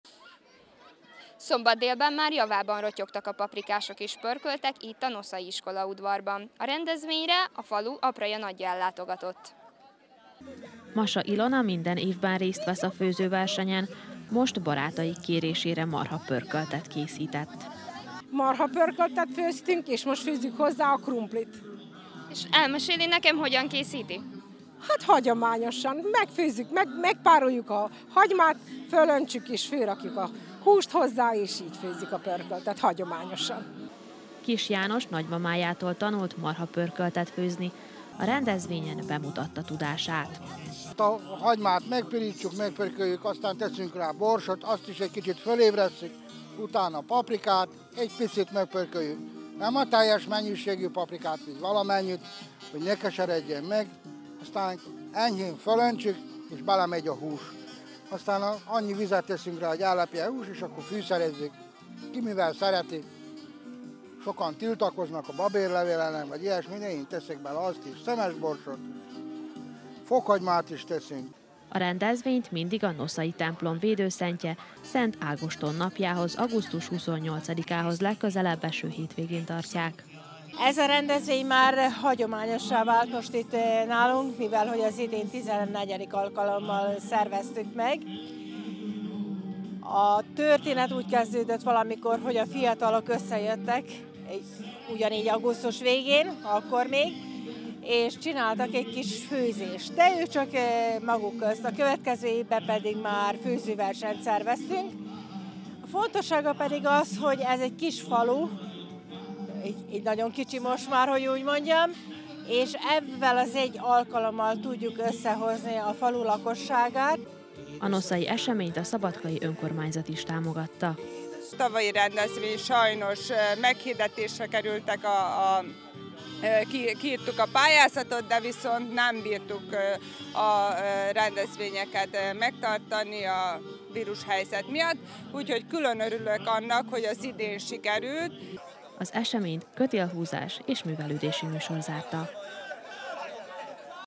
2021.09.04. - Fakanalat ragadott Nosza népe
A Nosza, hajrá süssünk, főzzünk elnevezésű rendezvényen 13 versenycsapat indult és mérettette meg főzőtudományát. Minden csapat szabadon dönthetett, milyen húsból készíti az ételt. A szakácsok körében a marha-, birka és csirkepaprikás volt a nyerő.